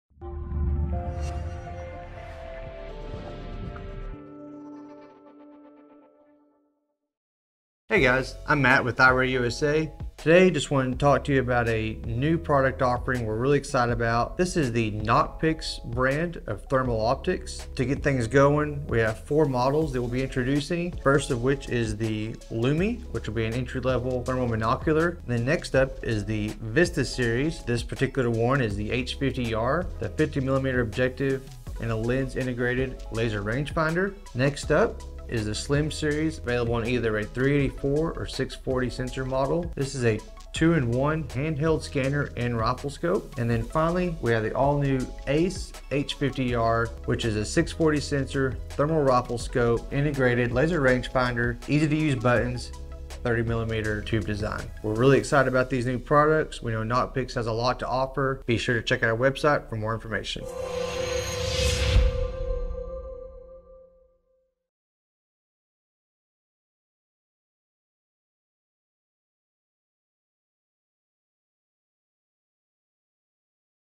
New Brand of Thermal Optics!
iRayUSA is proud to introduce an all new line of feature rich thermal optics to our customers. These initial offerings from Nocpix include an entry level monocular (LUMI), an advanced monocular with integrated LRF (VISTA), and two rifle scope options (SLIM and ACE). For more information on these various models